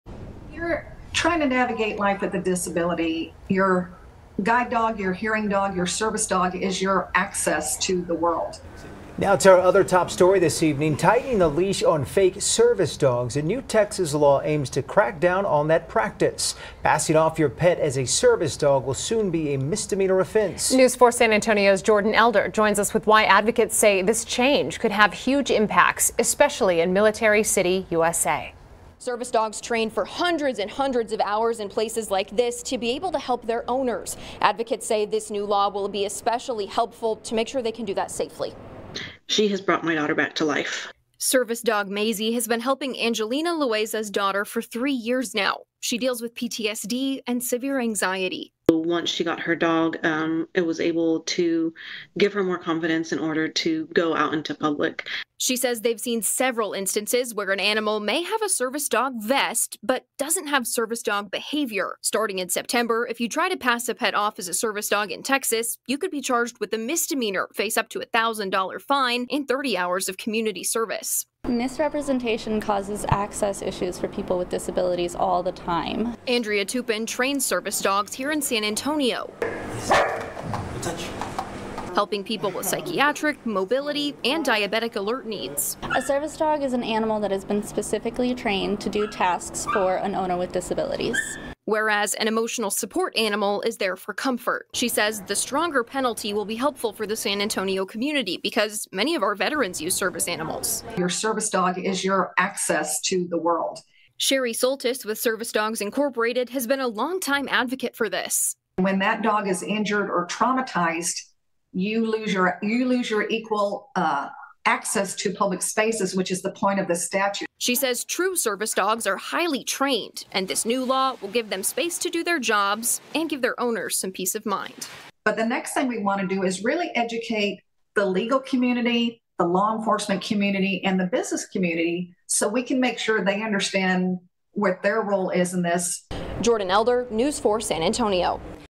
New Texas law says misrepresenting a service animal will land you in the dog house
People attempting to falsely pass their pets off as service animals will soon find themselves in the dog house. A new Texas law aims to crack down on the practice. It will soon be a misdemeanor offense.